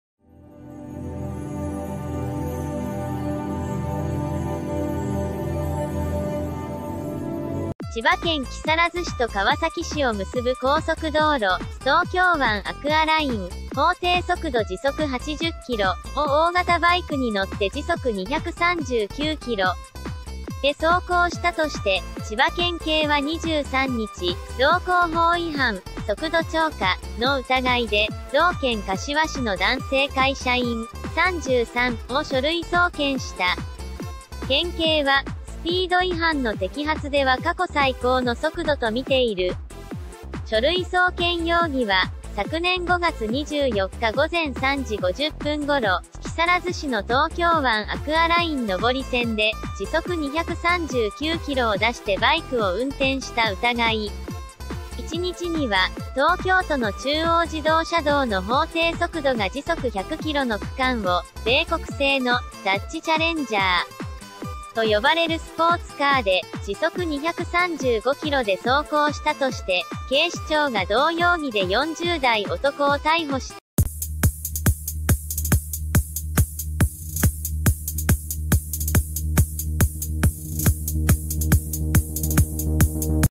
最新ニュース| 時速239キロで書類送検、スピード違反過去最速か - 社会 : 日刊スポーツ
千葉県木更津市と川崎市を結ぶ高速道路「東京湾アクアライン」（法定速度時速８０キロ）を大型バイクに乗って時速２３９キロで走行したとして、千葉県警は２３日、道交法違反（速度超過）の疑いで、同県柏市の男性会社員（３３）を書類送検した。県警は、スピード違反の摘発では過去最高の速度とみている。　書類送検容疑は、昨年５月２４日午前３時５０分ごろ、木更津市の東京湾アクアライン上り線で、時速２３９キロを出してバイクを運転した疑い。　１日には、東京都の中央自動車道の法定速度が時速１００キロの区間を、米国製の「ダッジ・チャレンジャー」と呼ばれるスポーツカーで、時速２３５キロで走行したとして、警視庁が同容疑で４０代男を逮捕した。（共同）